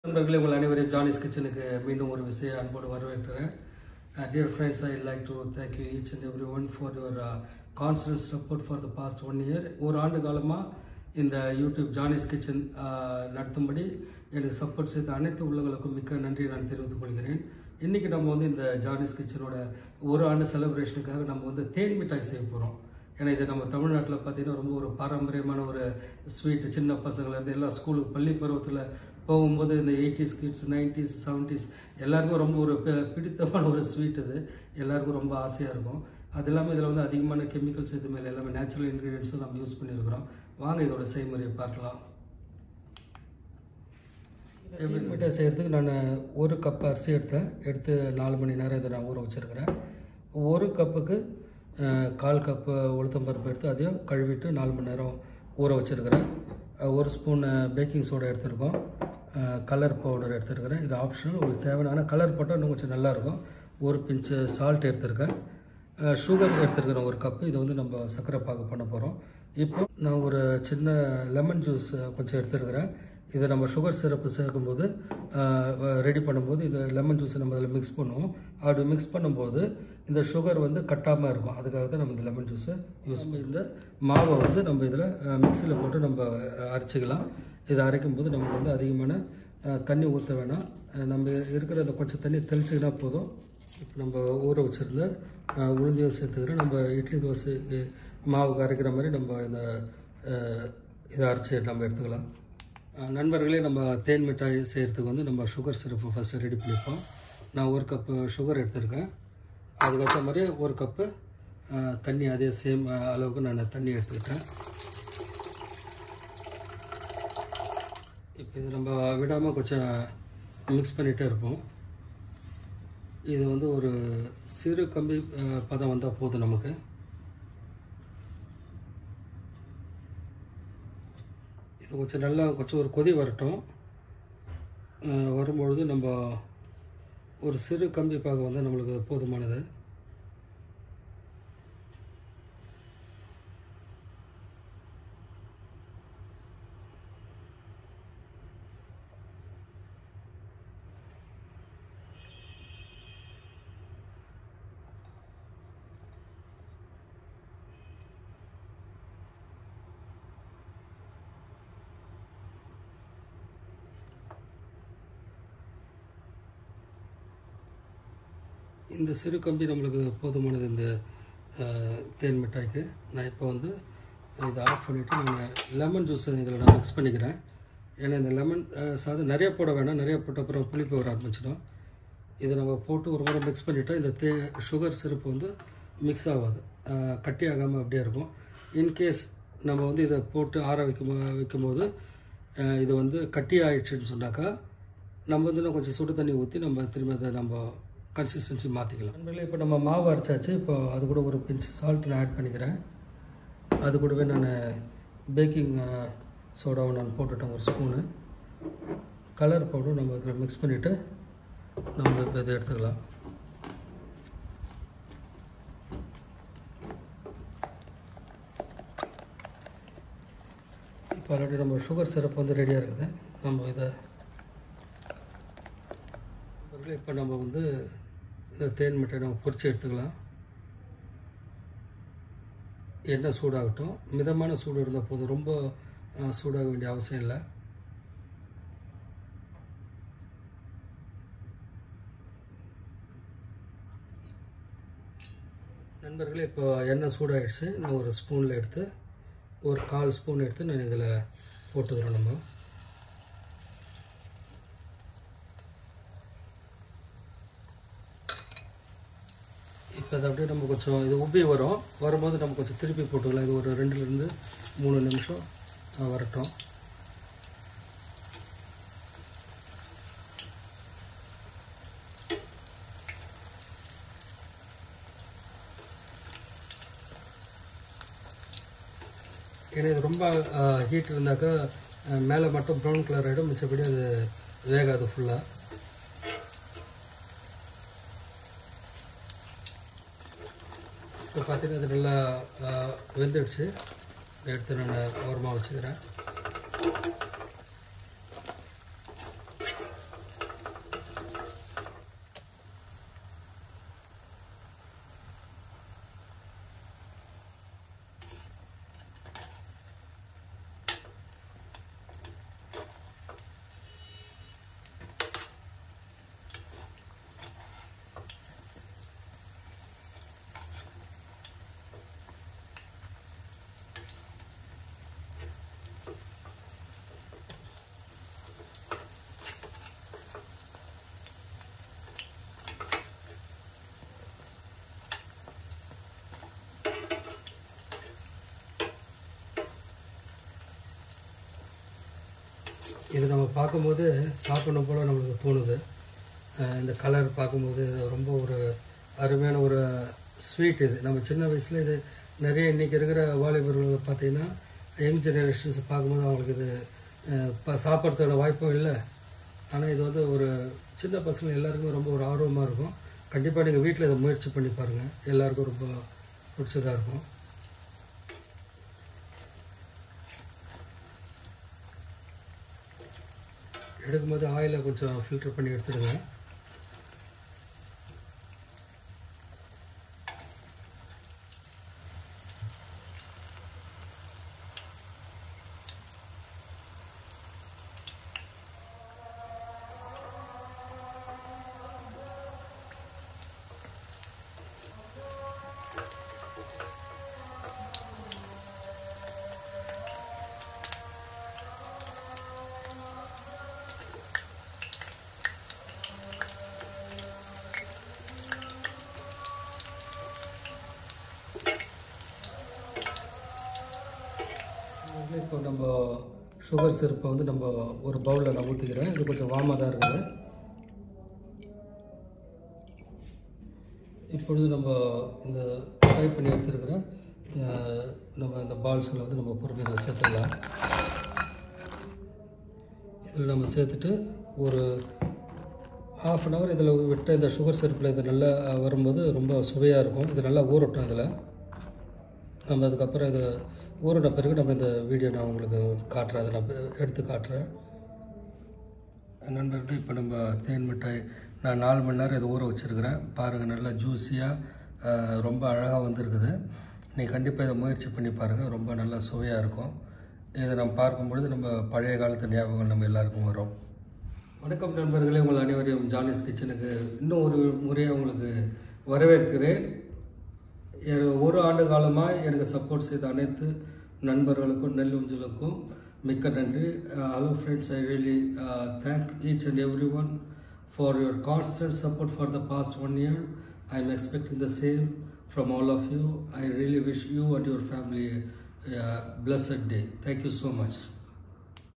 Then Mittai/தேன் மிட்டாய்/Honey Candy/90’s Kids Snacks/sweet Recipe/Johnny’s Kitchen
Ingredients required:

Raw rice - 1 Cup (soak 4 hours)
Urid dhall - 1/4 Cup
Baking Soda - 1 Sp
Colour powder - as required
Sugar - 1 Cup
Salt
Lemon juice